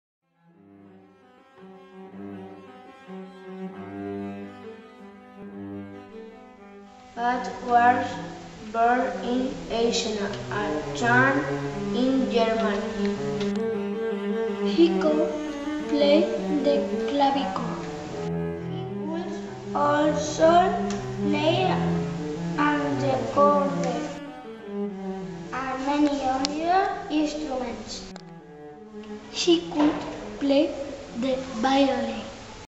C.E.I.P. ENRIQUE RAMBAL.3RD A ENGLISH TEAM 1
A BRIEF STORY ABOUT BACH, TOLD BY THIRD GRADERS